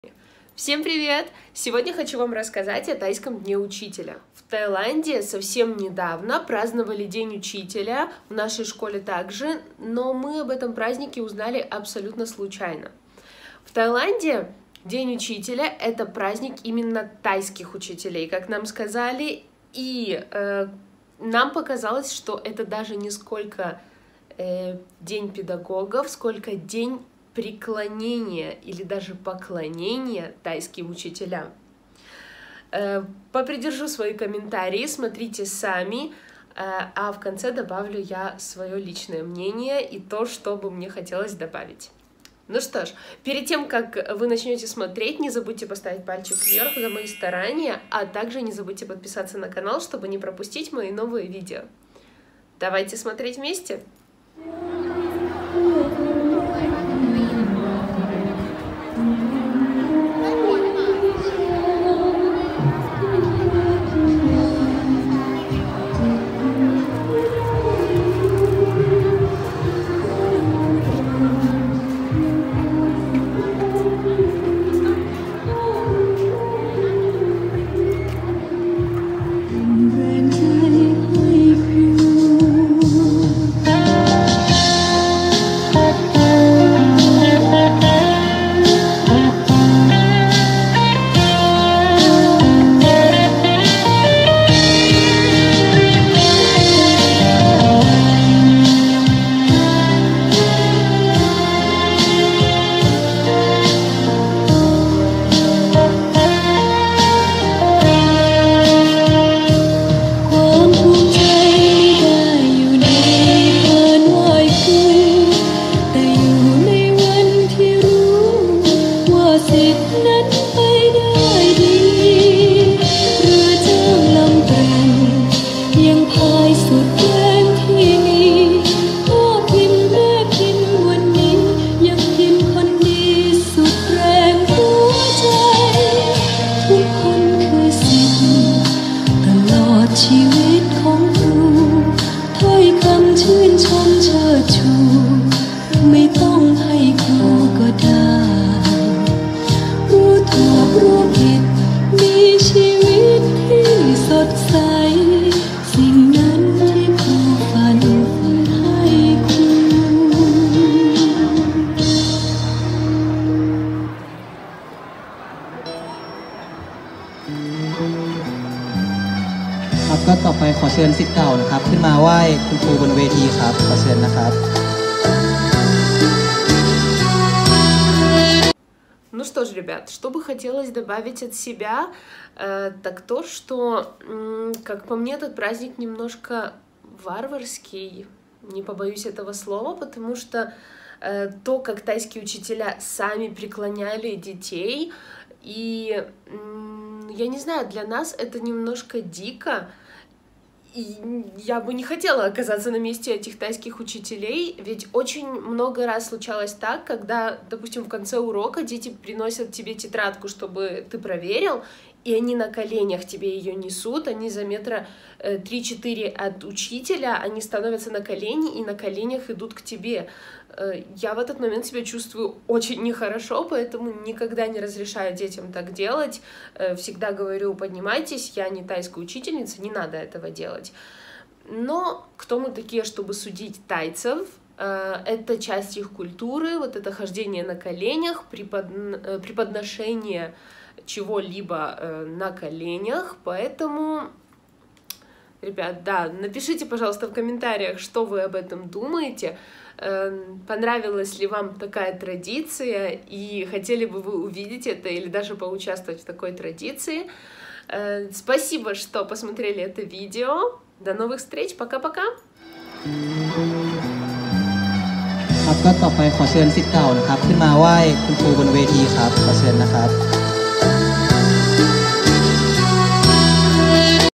ПОКЛОНЕНИЕ УЧИТЕЛЯМ или ТАЙСКИЙ ДЕНЬ УЧИТЕЛЯ 👩‍🏫 | Таиланд
Всем привет!
Меня зовут Марьяна и это мой блог о жизни и работе в Таиланде.
В этом видео я хочу Вам показать как празднуют День Учителя в Таиланде.